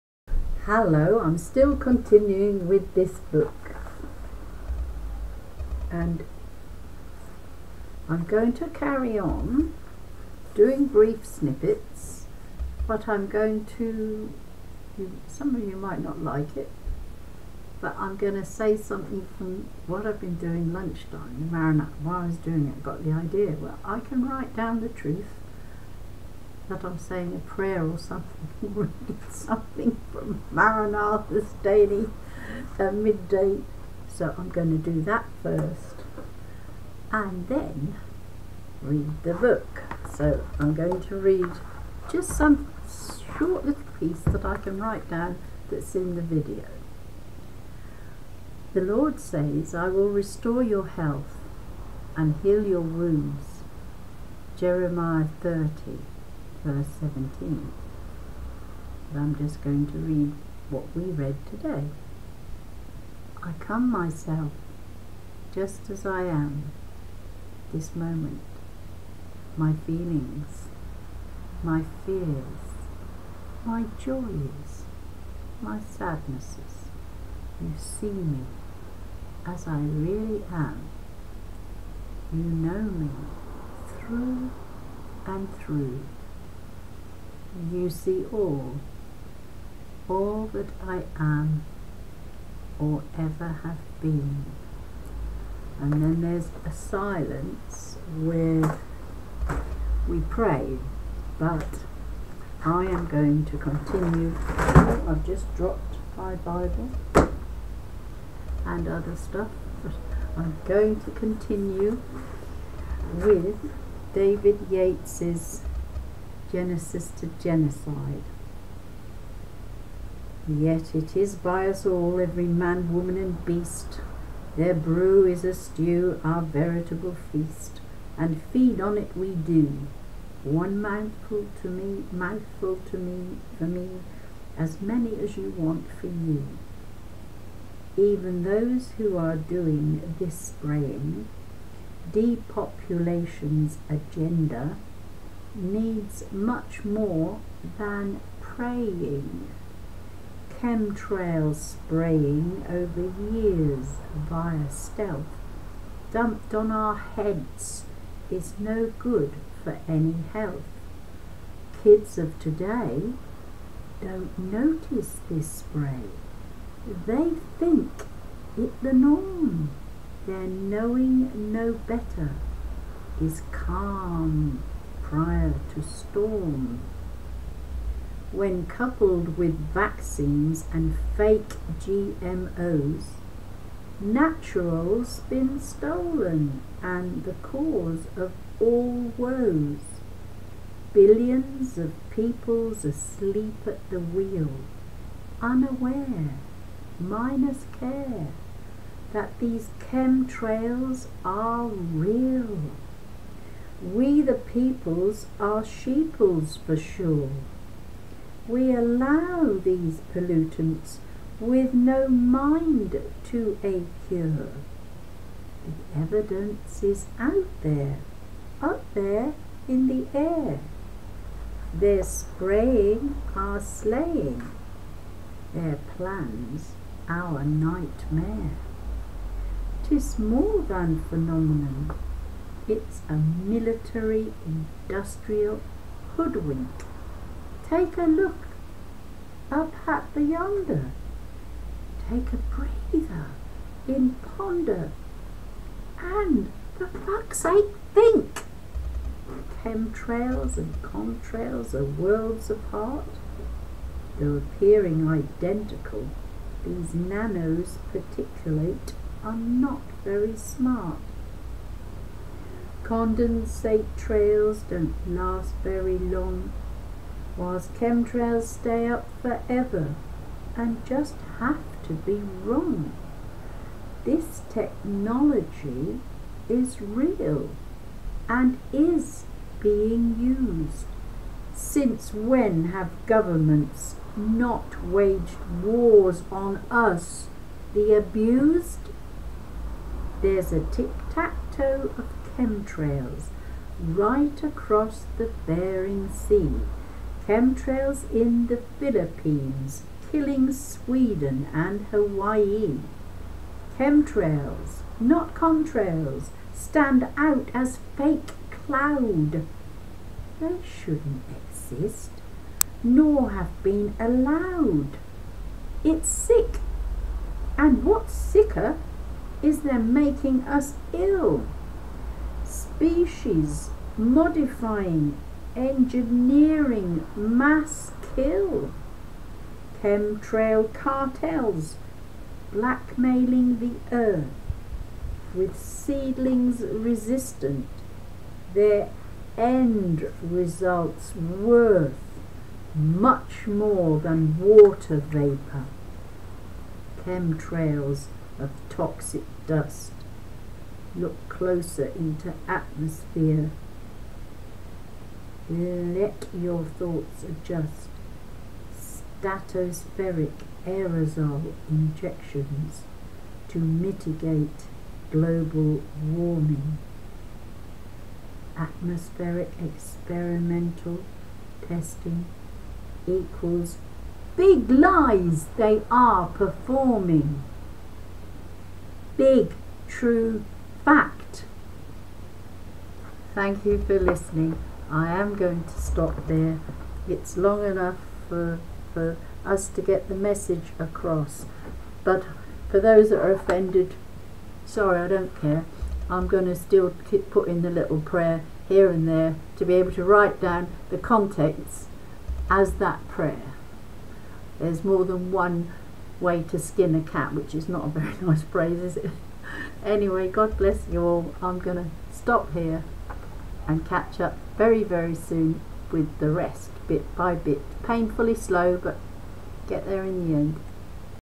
GENESIS to GENOCIDE NATURAL STOLEN by DAVID YATES 19 AUG 2020
The Lord says:'I will restore your HEALTH and HEAL your wounds.' Jeremiah 30:17. I come as myself.Just as I am this moment. My feelings, my fears, my Joys, my sadnesses. You see me as I really am. You know me through and through. You see all. All that I am. Or ever have been.
If you do not want to receive the MESSAGE then be it on your own Head when the shit HITS the FAN. The Trumpeter is indeed DAVID YATES not me. Mine is the VOICE HIS the WORDS.